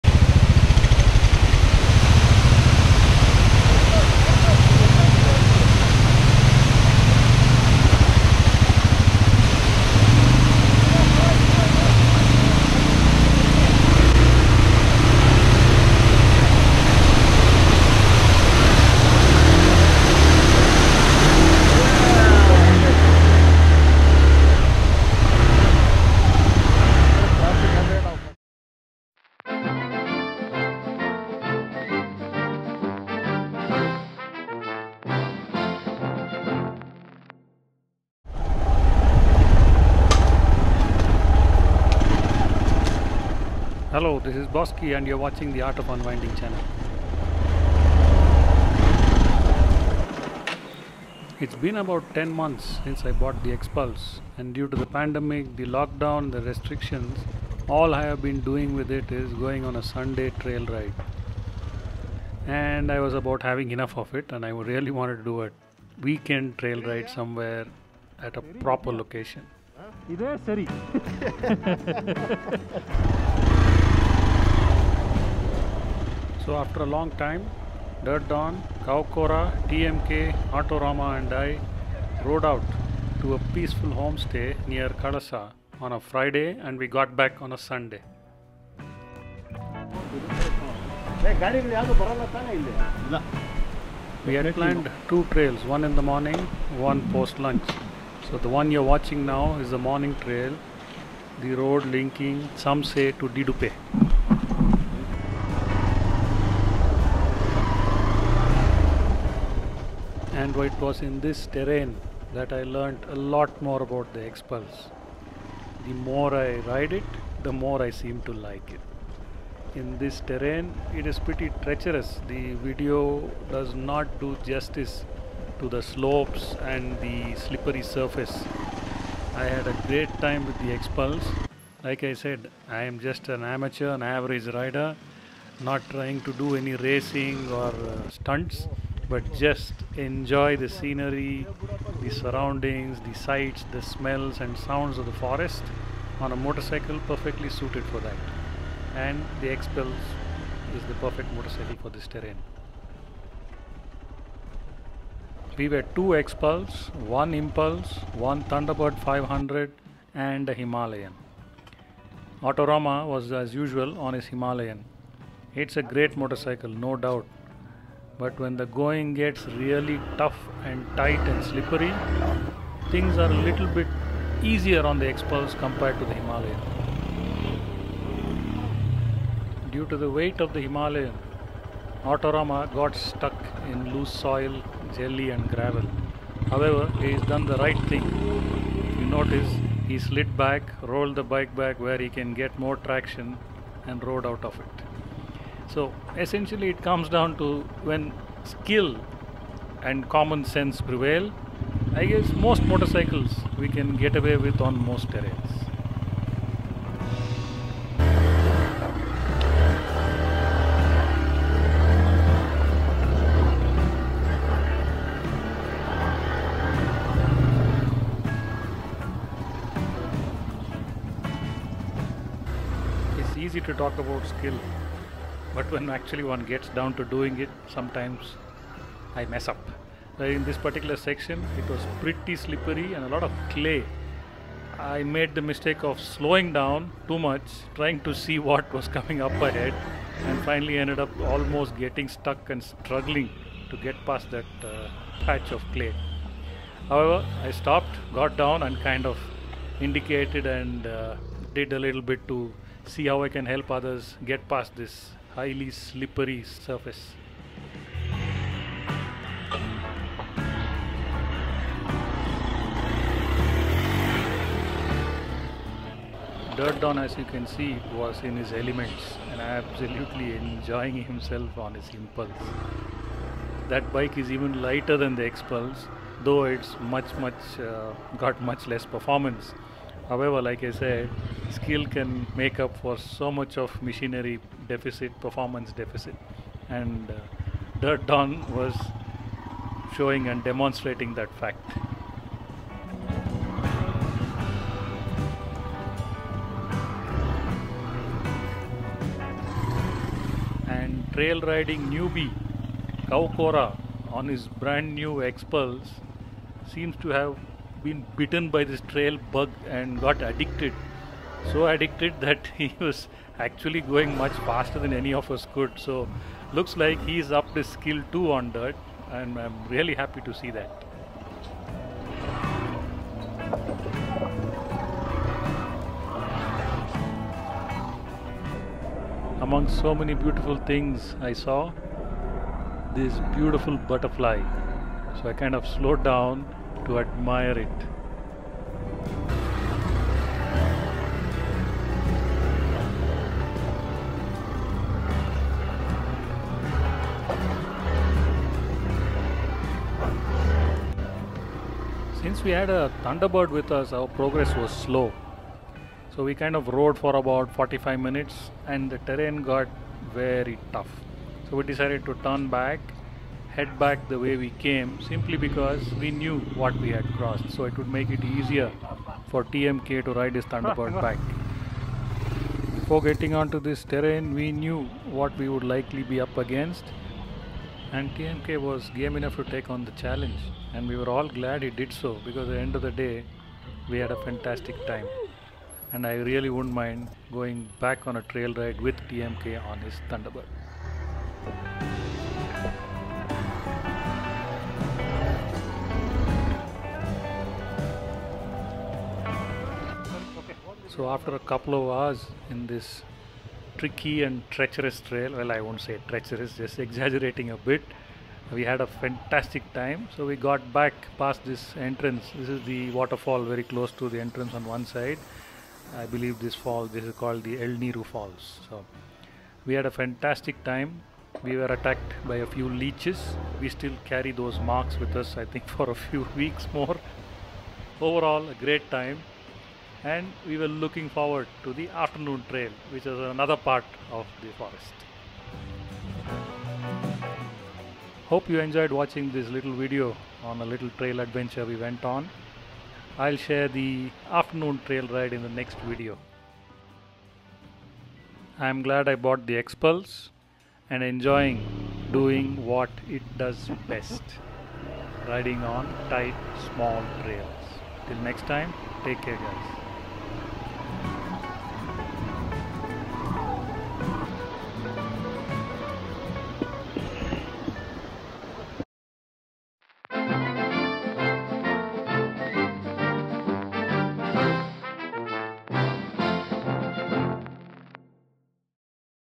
Xpulse 200 | Exploring Kalasa Trails | Samse to Didupe
Hello! After 10 months of ownership and having been forced to ride only in and around Bangalore due to the pandemic, it was a huge relief and great fun to go out on a weekend overnight trip to Kalasa with the Xpulse.

And, this ride reinforced the feeling that the more I ride the Xpulse, the more I am liking it. This motorcycle is serving me well and appears to be tailor-made for the purpose I bought it for. I am indeed one happy Xpulse Owner. Although the fact remains that the performance is a bit of a pain on open highways.

Hope you enjoy this video where 5 of us went exploring into the forests between Samse & Didupe.

Cheers!